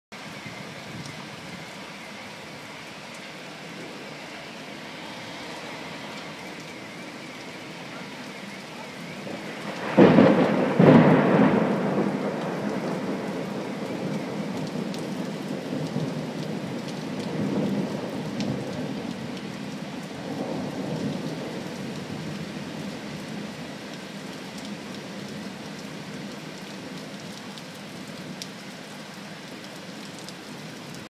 Crazy thunderstorm
One crazy thunderstorm rolled through the Bronx. I just happened to have the camera rolling before the biggest lightning strike occurred.